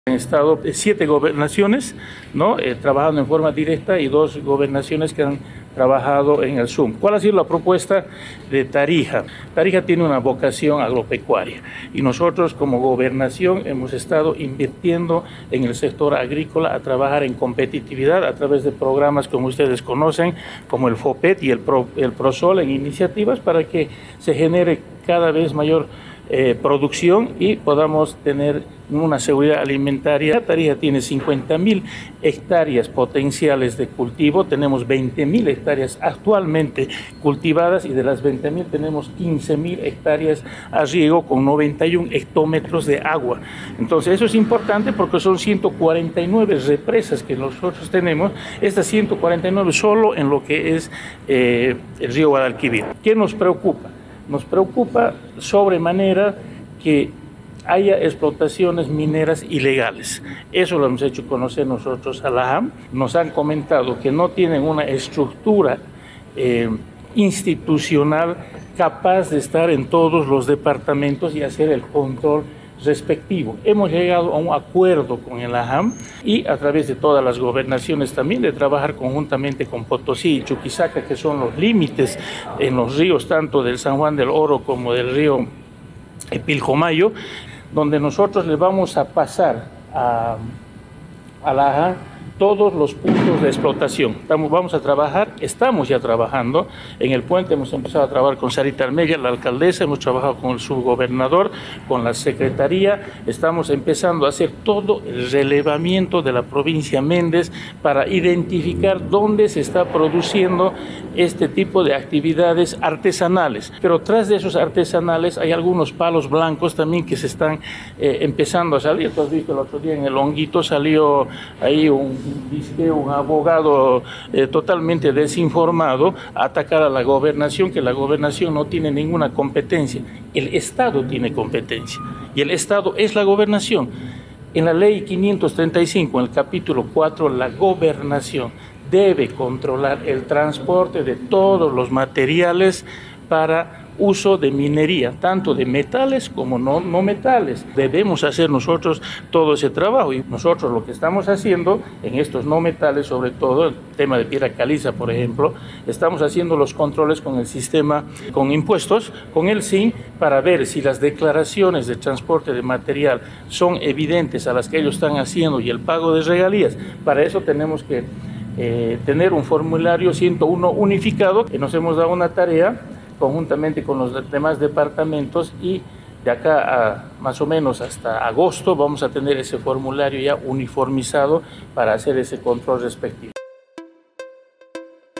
Gobernación aborda minería ilegal en el XIII Encuentro del Consejo Minero en Pando
El Gobierno Autónomo Departamental de Tarija participó en el XIII Encuentro del Consejo Minero de Gobernaciones (COMINGOB) en Pando, enfocado en la creación de una normativa para controlar las cooperativas que operan ilegalmente en el departamento.
El Ing. Efraín Rivera, Secretario de Desarrollo Productivo, Recursos Naturales y Medio Ambiente, destacó que las gobernaciones deben regular la minería para evitar la exploración irracional conforme a la ley.